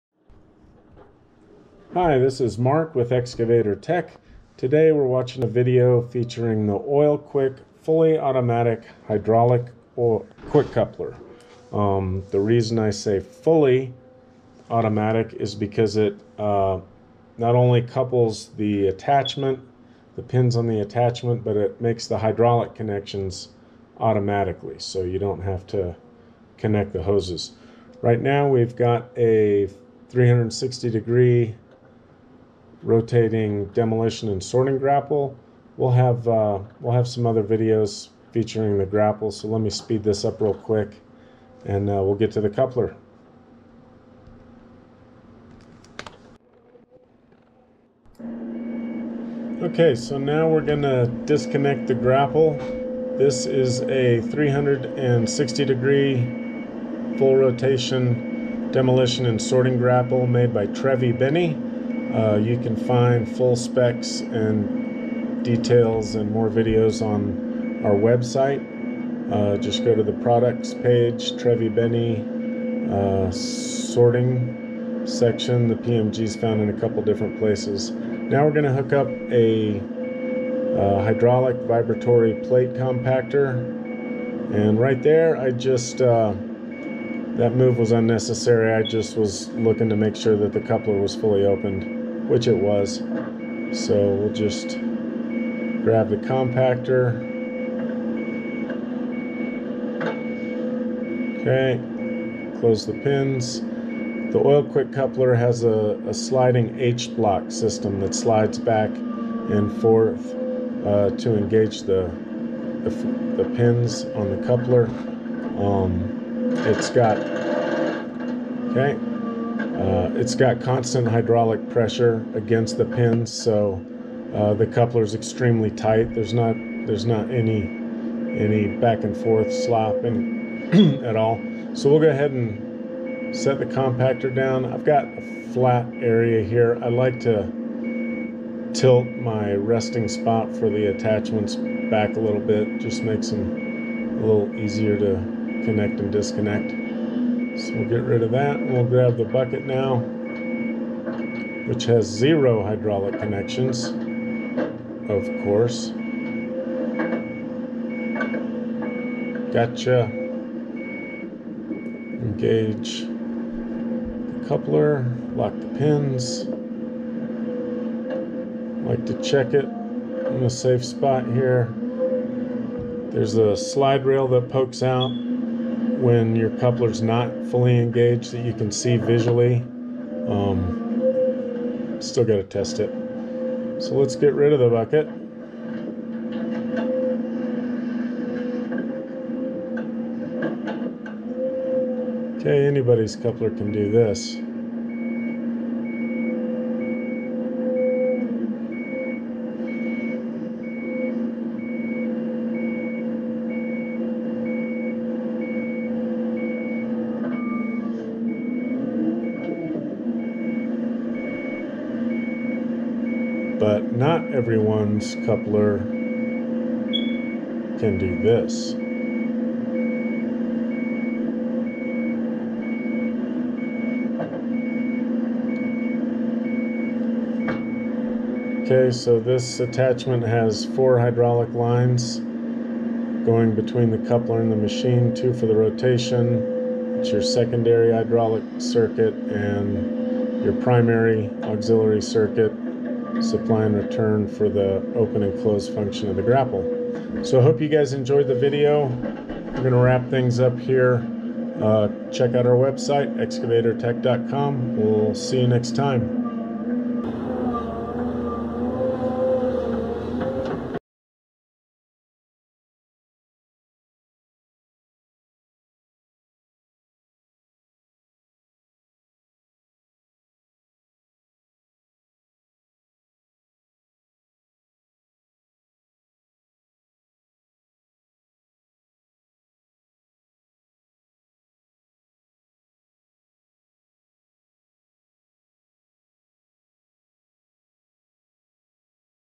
Excvator Tek: Oil Quick video featuring Volvo EC290, Trevi-Benne PMG 20 grapple, compactor, bucket
Oil Quick couplers for hydraulic excavators eliminate the need to manually connect and disconnect hydraulic lines between your excavator and it's attachment.  Work fast and safe.  This technology is robust and field tested since 1994 (primarily in Europe).  Contact ExcavatorTek.com for more information and a quote to equip your machine with the technology to do more faster and safer than your competitors.